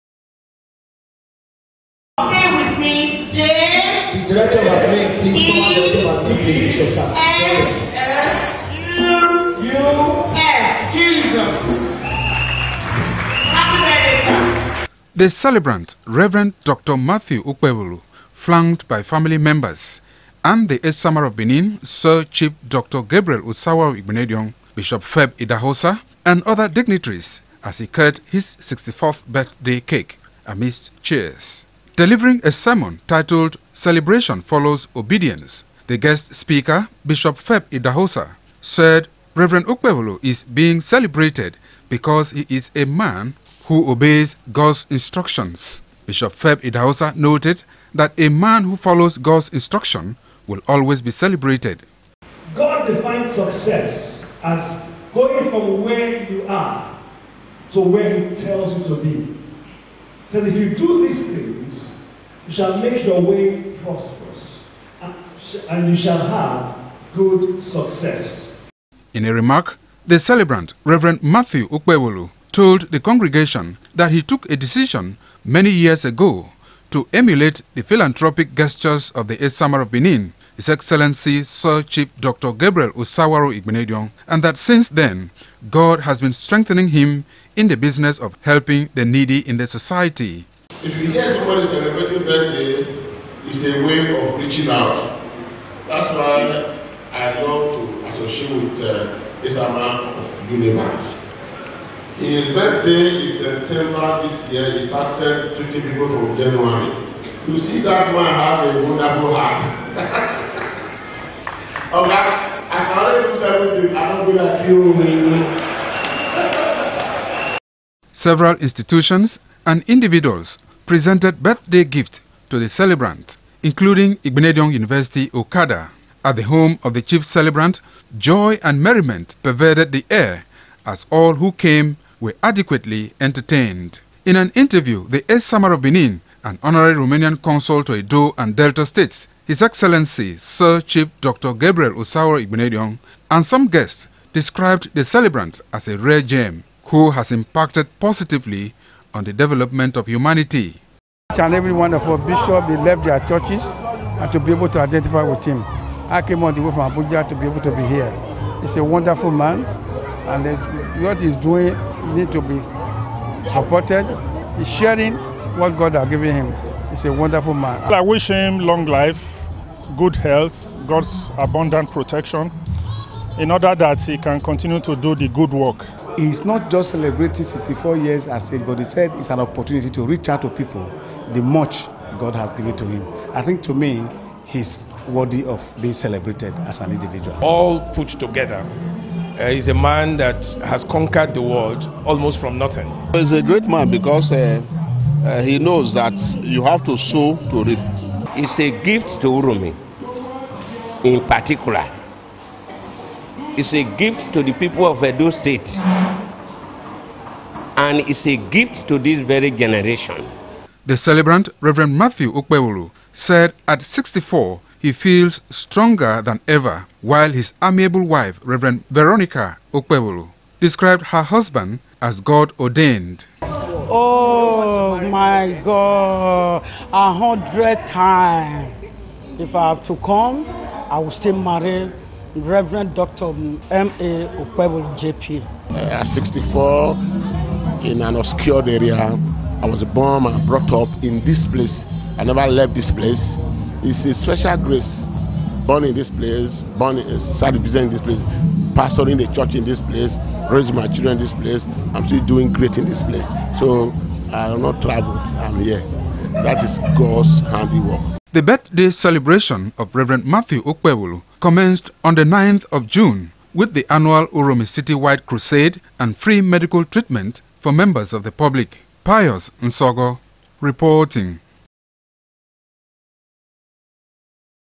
Rev. Dr. Matthew Okpebholo celebrates birthday in style
The 64th birthday celebration of the Managing Director, Ray Royal Construction Company, Reverend Matthew Okpebholo climaxed Sunday at the Church of God Mission International Inc., Archbishop Benson Idahosa Cathedral, Uromi.